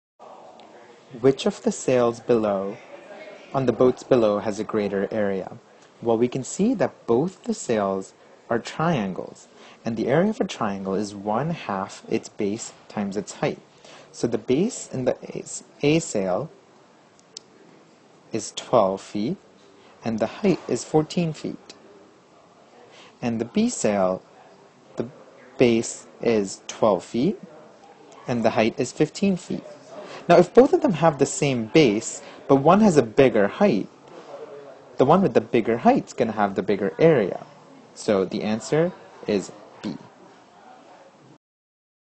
[6.G.1-2.5] Polygon Areas - Common Core Standard
Apply the area formula for other triangles to solve real world problems